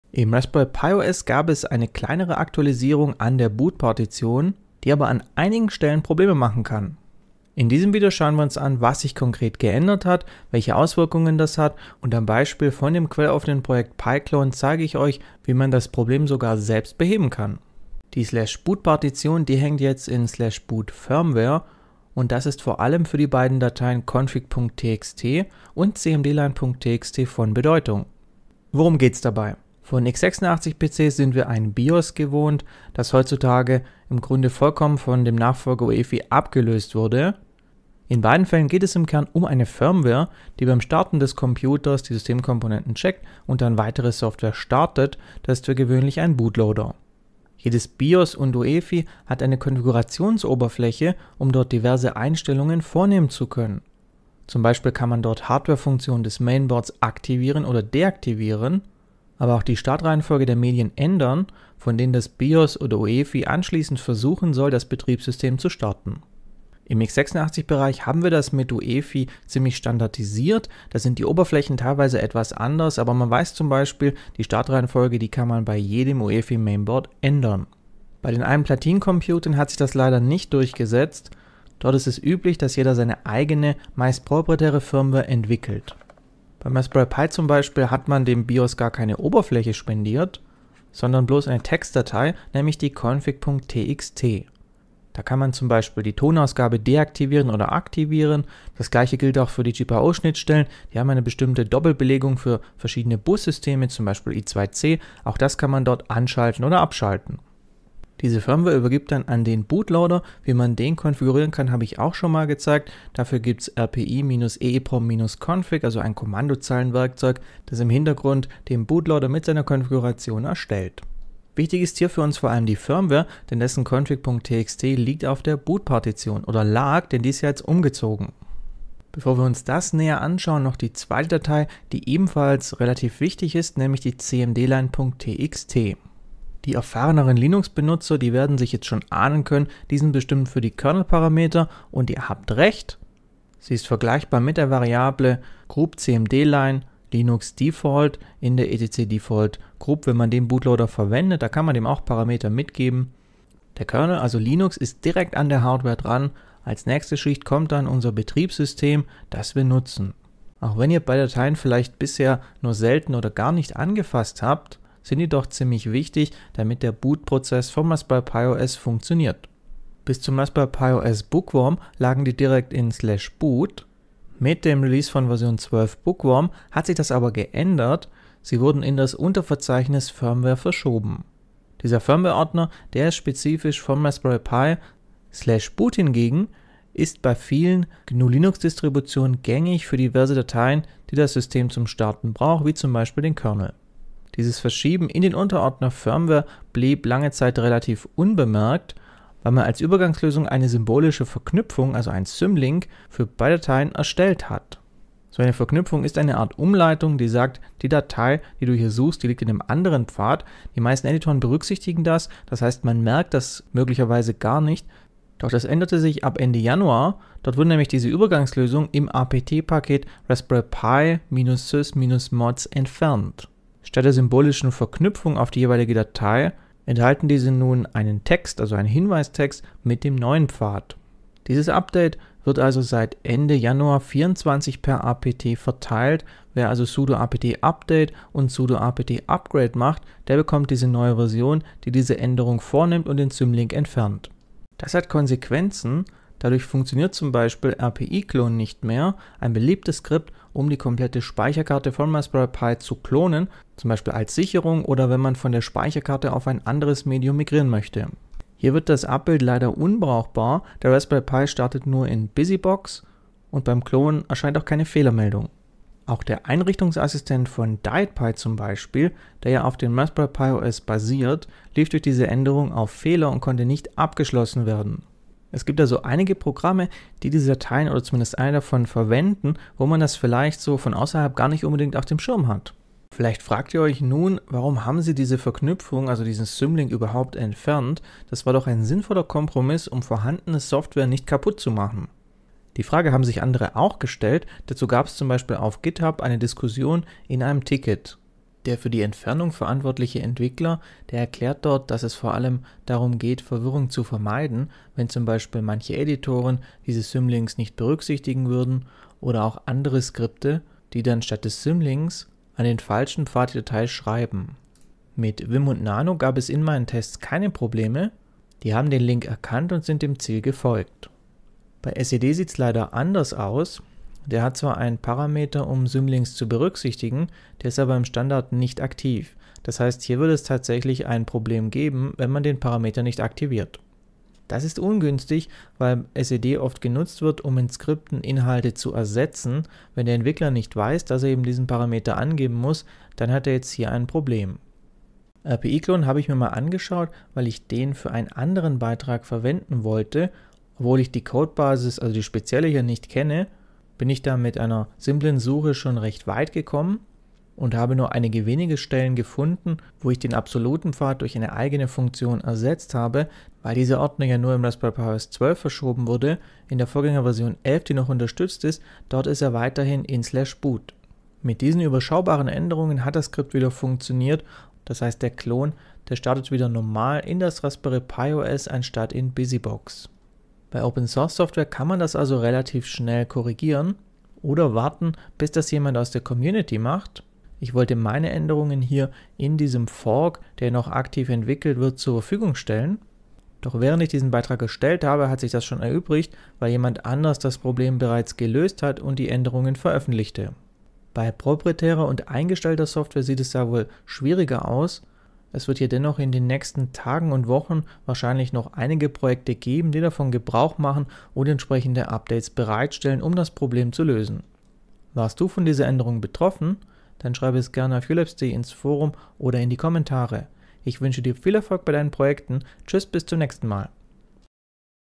Kleine Raspberry Pi OS Änderung, große Probleme: Das hat sich in /boot geändert!
Im Raspberry Pi OS 12 wurde recht unbemerkt die Ordnerstruktur in /boot verändert. Ein weiteres Update beschädigt u.a. rpi-clone, DietPi und weiterer Software. Hier erfährst du, welche Änderungen es gab. Außerdem zeige ich an rpi-clone, wie man das Problem bei quelloffener Software selbst lösen kann

=== Inhalt ===
00:00 Was hat sich mit Raspberry Pi OS 12 in /boot geändert?
03:14 Warum die Probleme erst jetzt auftreten: Das kleine Paketupdate & seine Folgen
05:50 Ich repariere rpi-clone